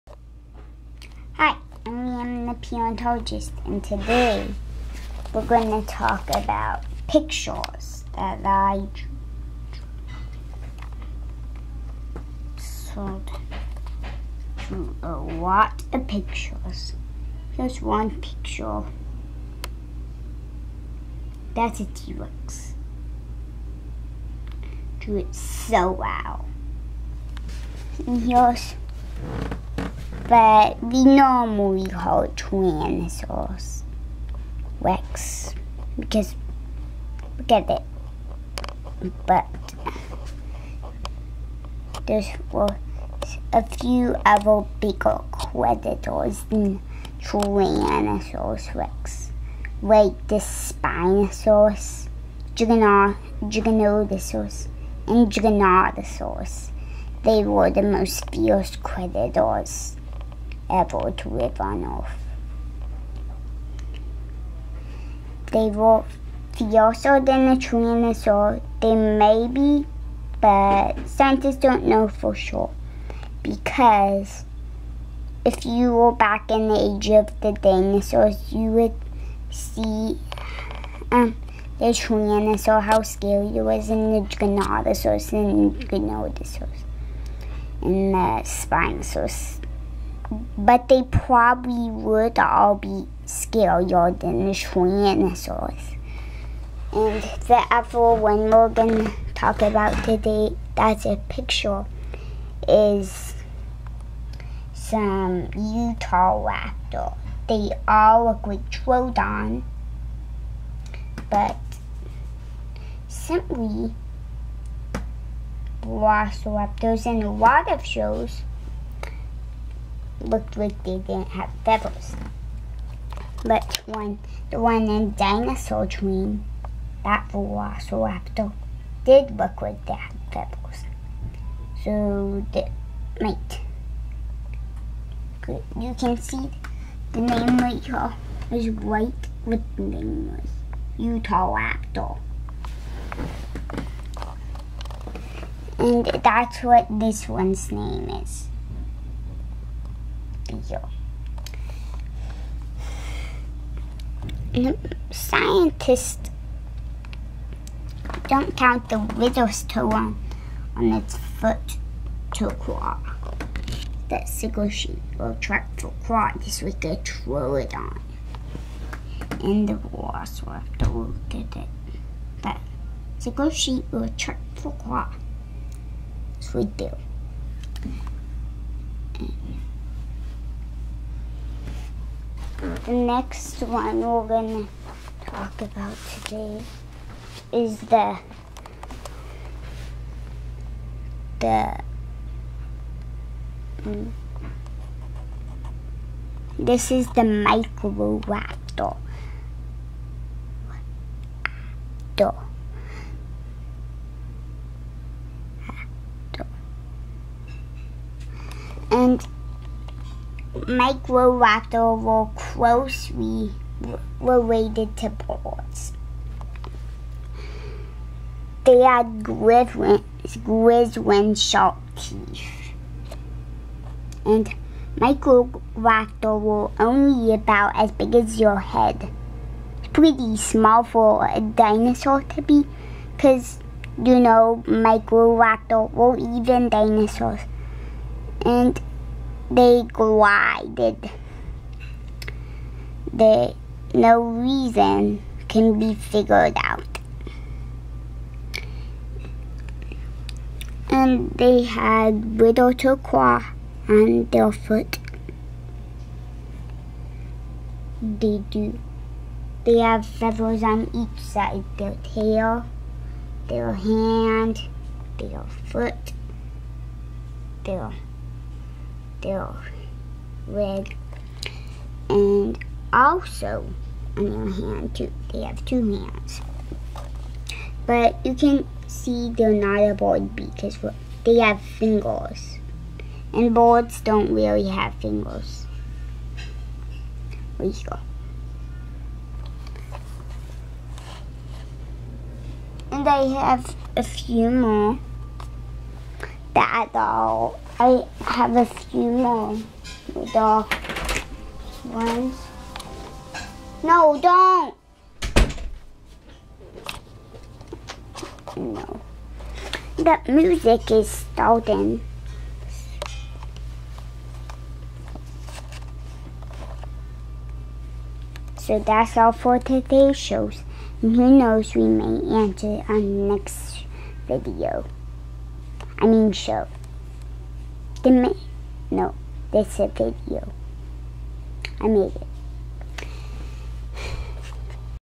T-Rex, Microraptor & Velociraptor - Liam The 5 Yr Old Paleontologist
Liam the 5 yr old paleontologist talks about the T-Rex, Microraptor & Velociraptor.